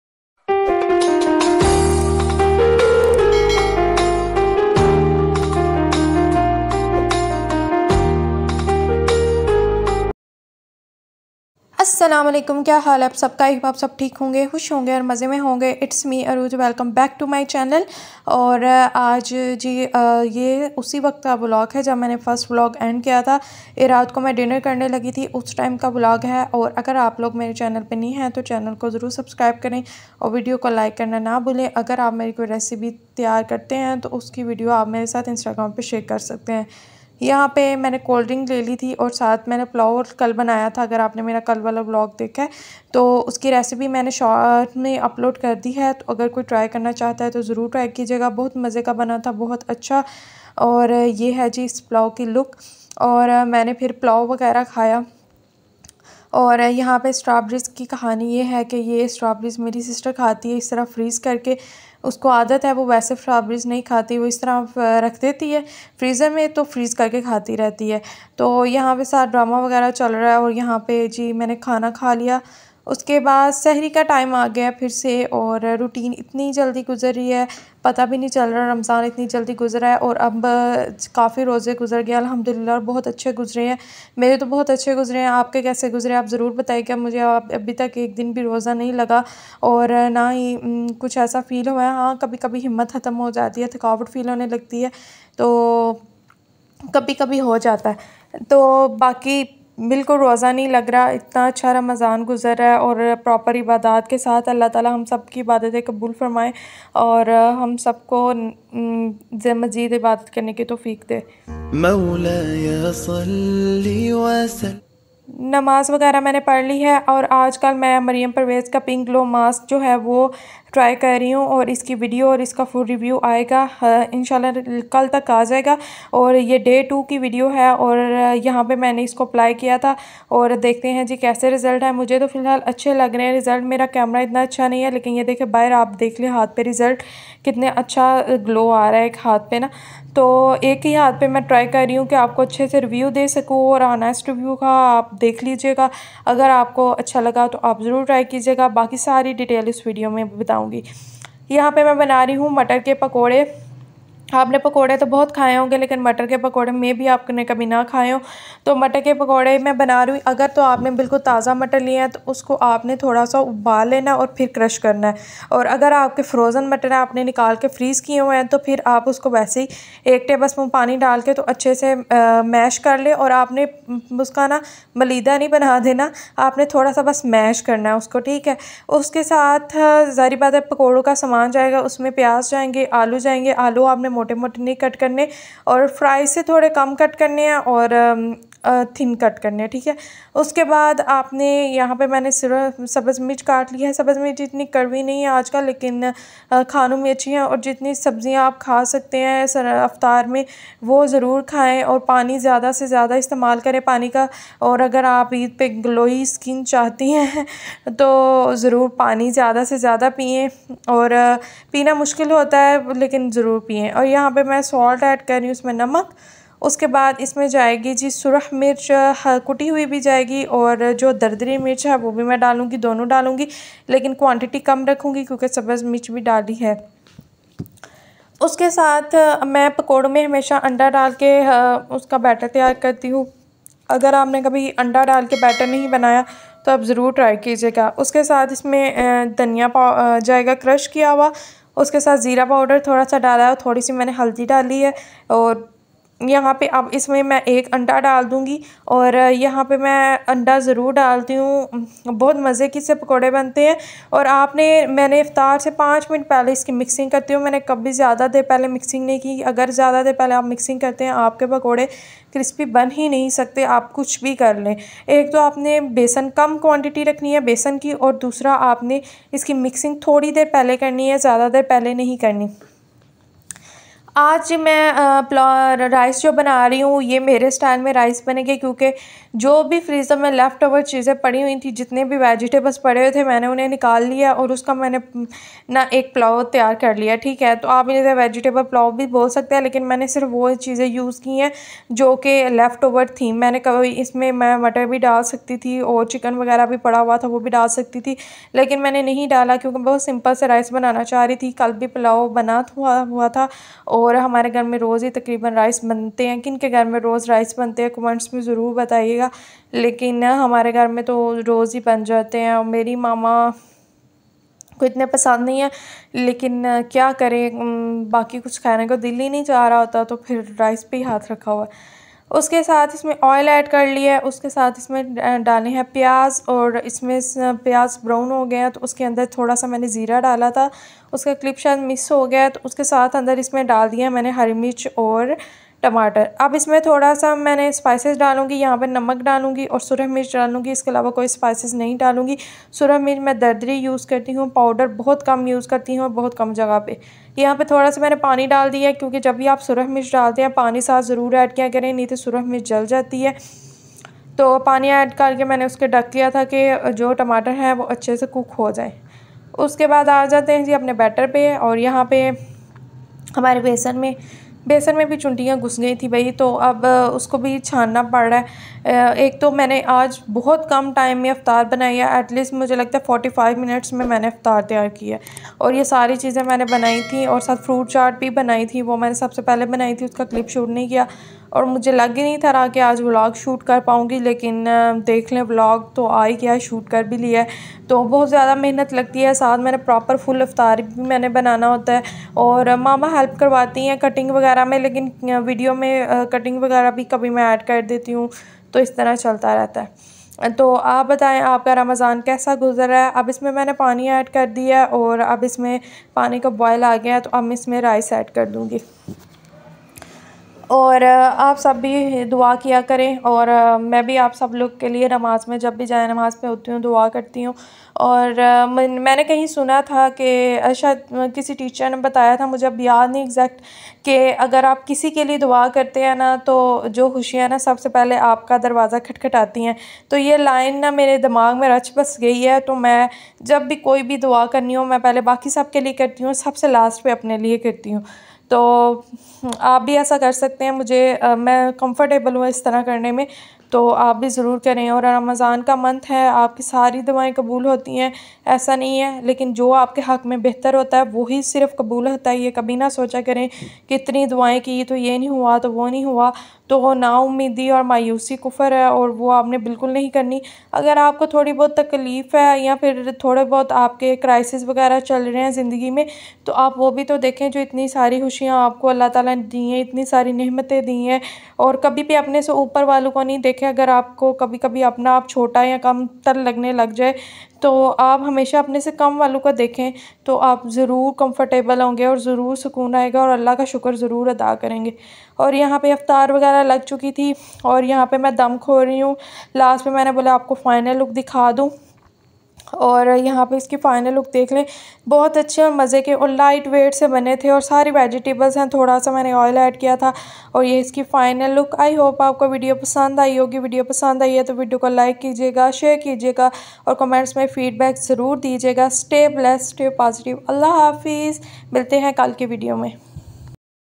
How to make peas pakora| Watermelon face pack 🍉_Arooj Fatima
Today my suhoor and iftar routine 🙂